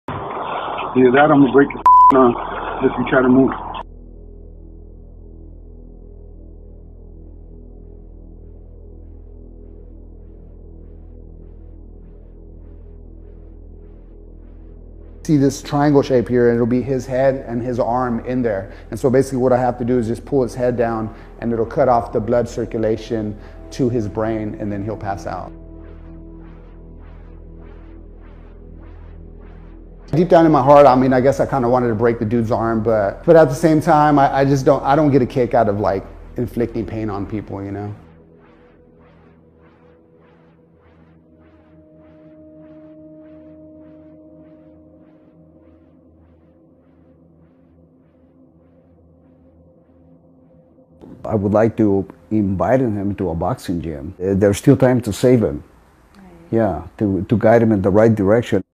Alleged intruder taken down by MMA fighter
"I could have broken his arm easily, but I don't like to see people get hurt."

Would-be burglar taken down by MMA fighter, authorities say